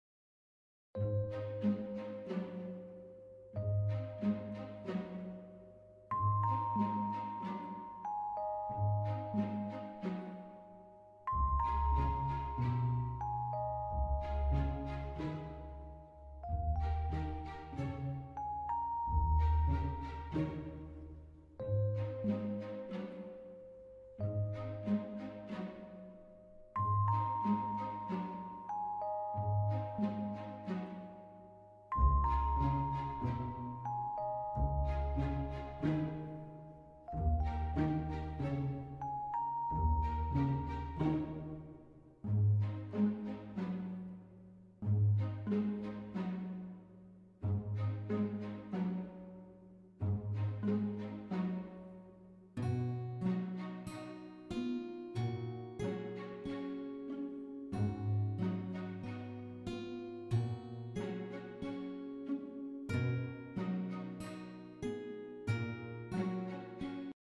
Izuku doesn’t want Hisashi to go to work🥺💕|| Younger Midoryia|| Inko x Hisashi
Anime characters used:
Izuku Midoryia
Inko Midoryia
Hisashi Midoryia